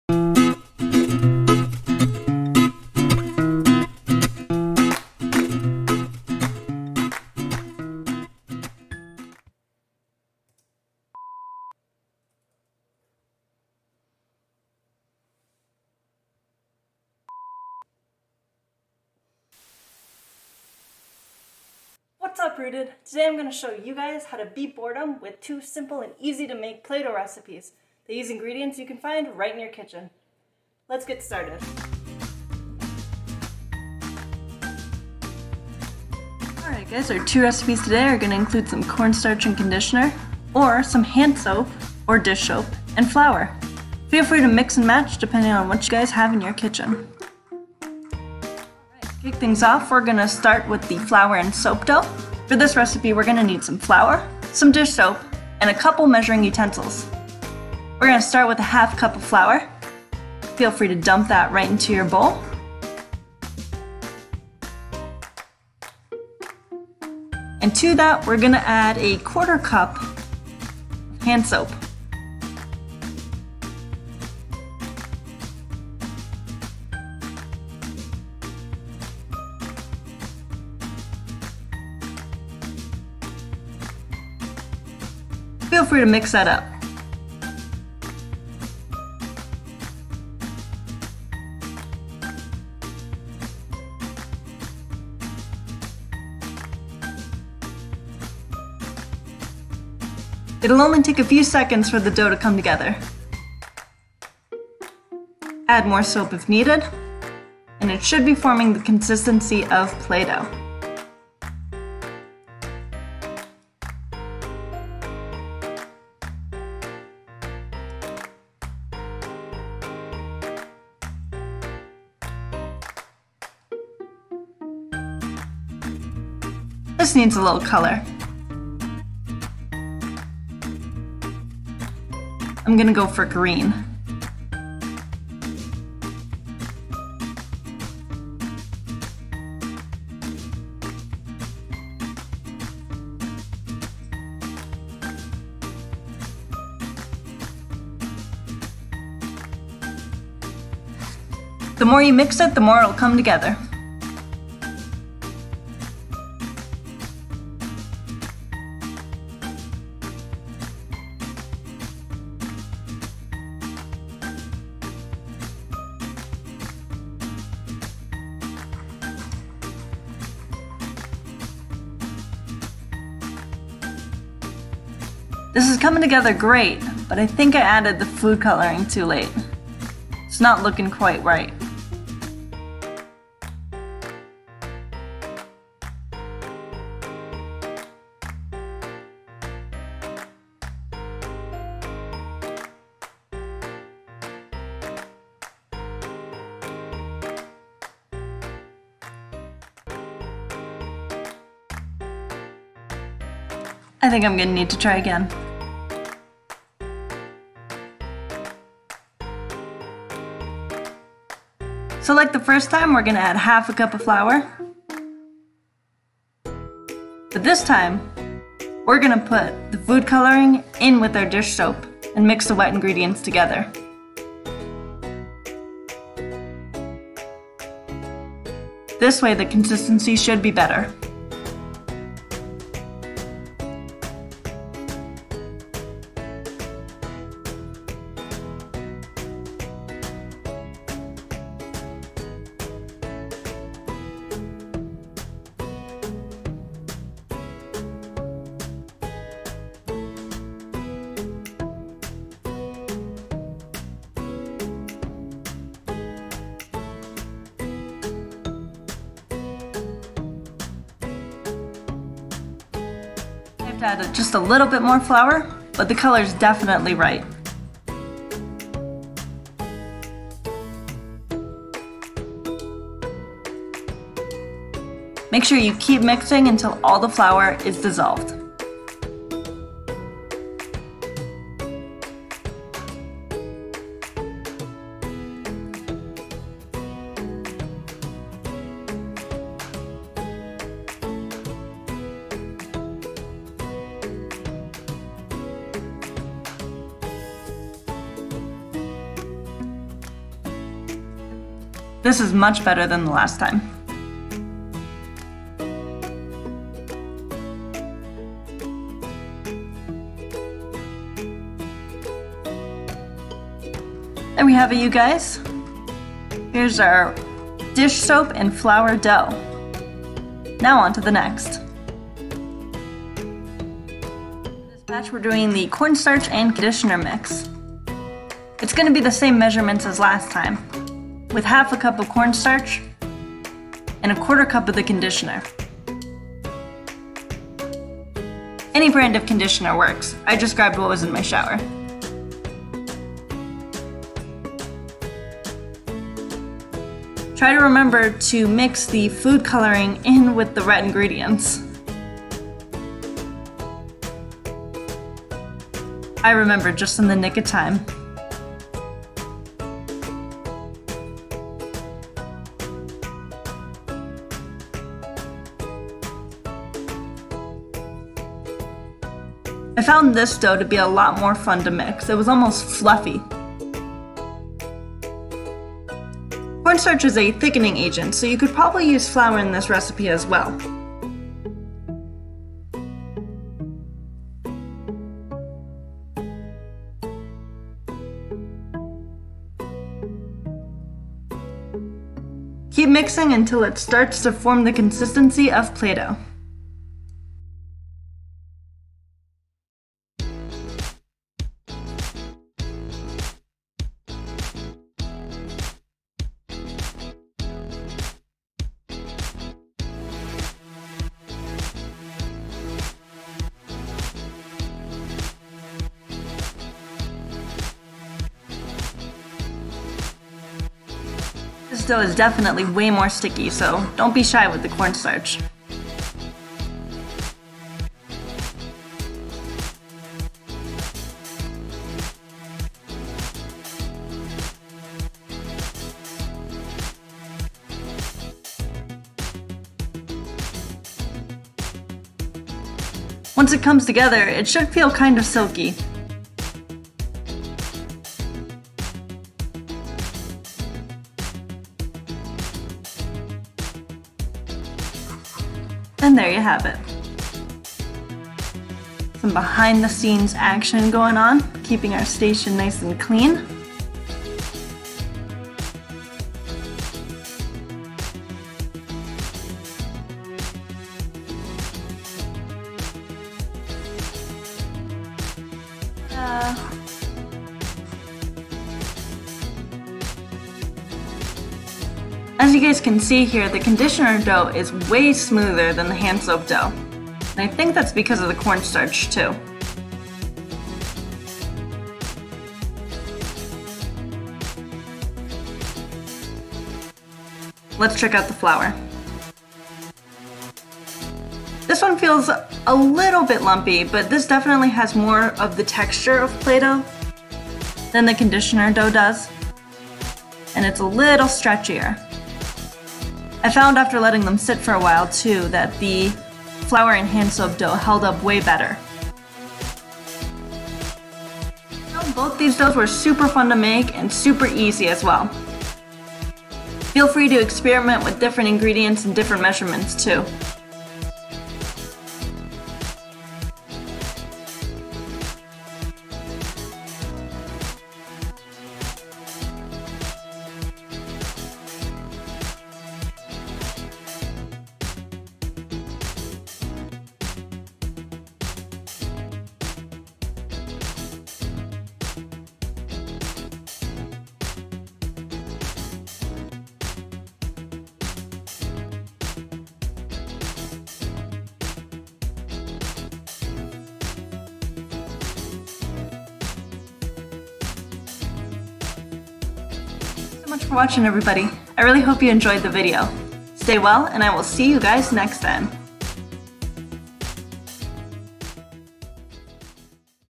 Playdough 2 Ways
The RootEd educators, as part of the Empire State Afterschool program, in partnership with the City of Binghamton School District, lead youth through an experiment in creating homemade playdough using ingredients you can find in your house. Watch the experiment and have some fun!